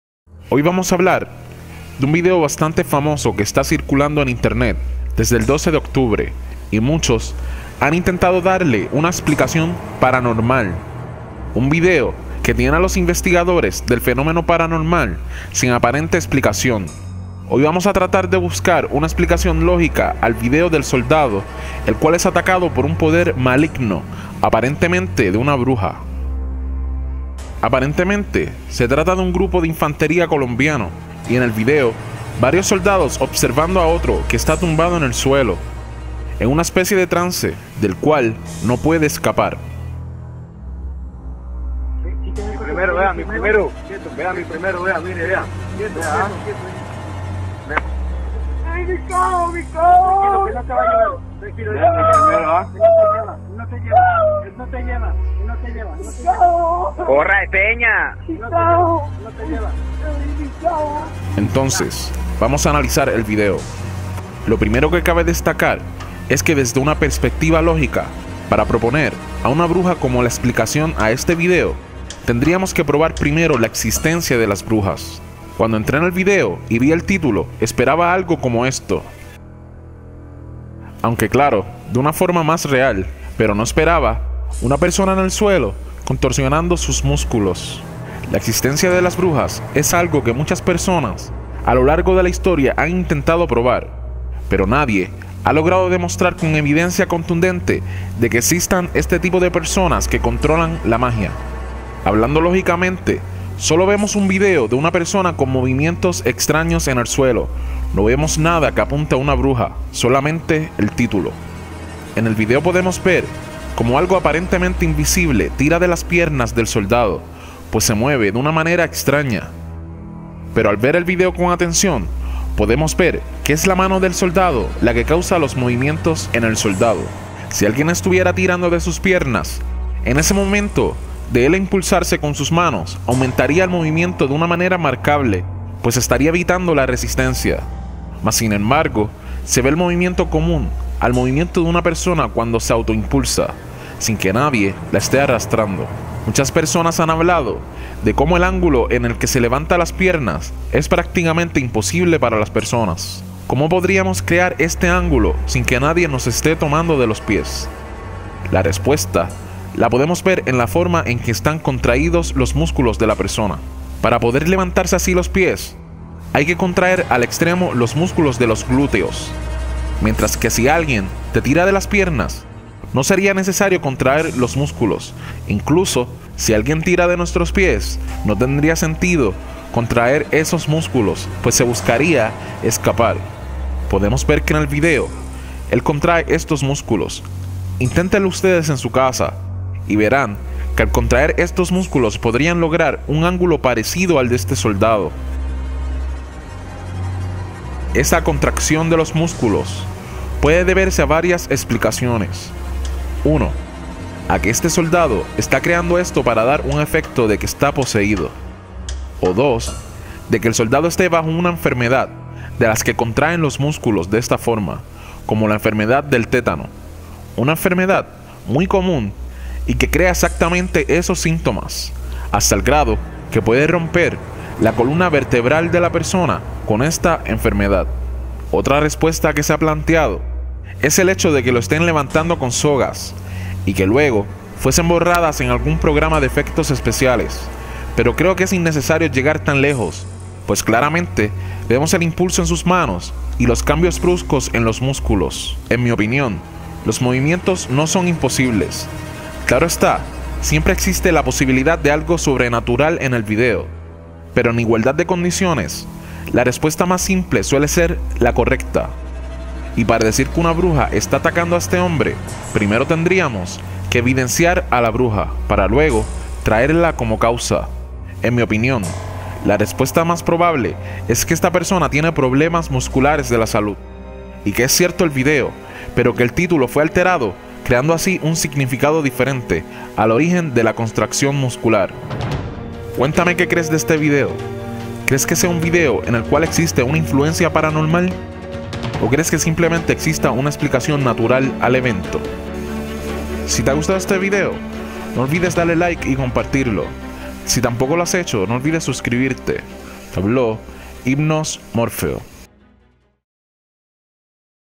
La Verdad de la Bruja Que ataca un Soldado | Análisis Investigativo
Hoy analizamos el video de la Supuesta bruja que ataca a un Soldado colombiano, y intentados darle una explicación lógica a este misterioso vídeo

Video Original

Muchas Gracias por pasar no olviden subscribirse